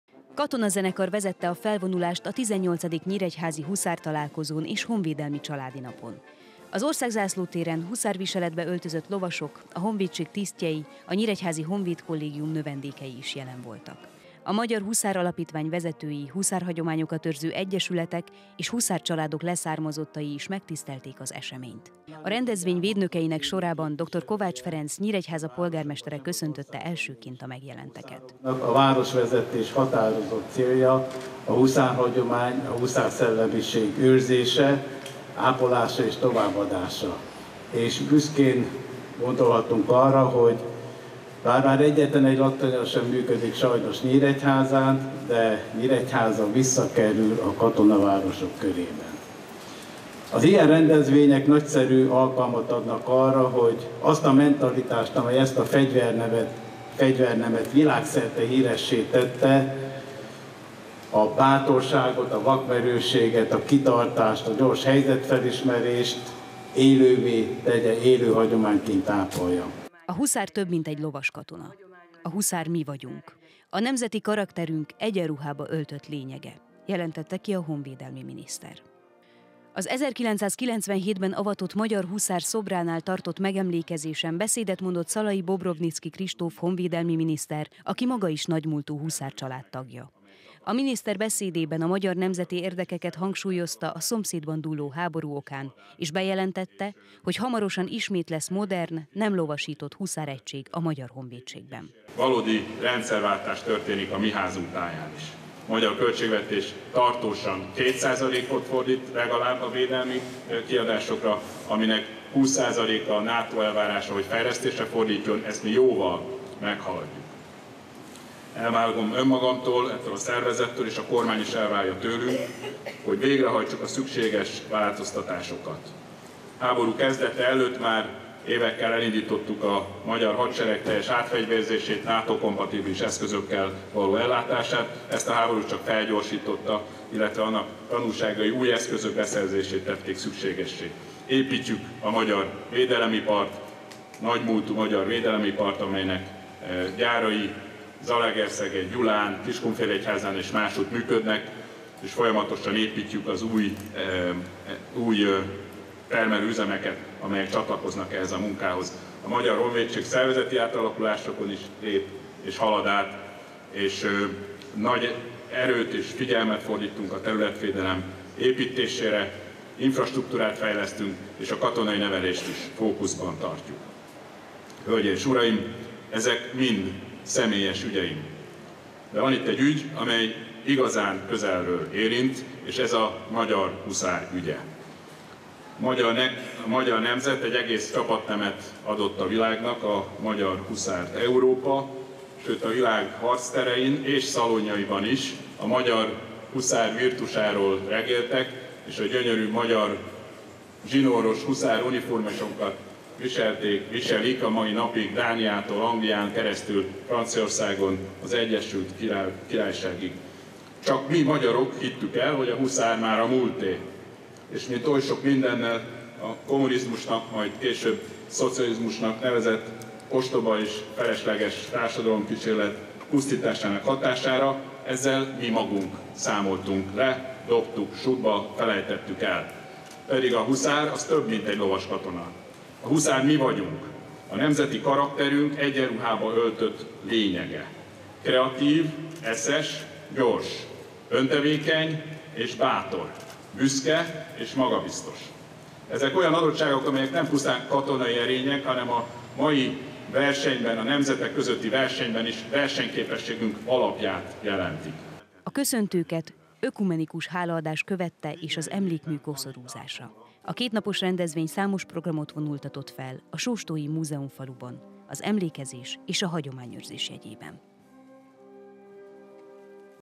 Huszártalálkozó Nyíregyházán
” A huszár több, mint egy lovas katona, „a huszár mi vagyunk", a nemzeti karakterünk egyenruhába öltött lényege - jelentette ki a honvédelmi miniszter szombaton Nyíregyházán. A megyeszékhelyen tartották a 18. Huszártalálkozót.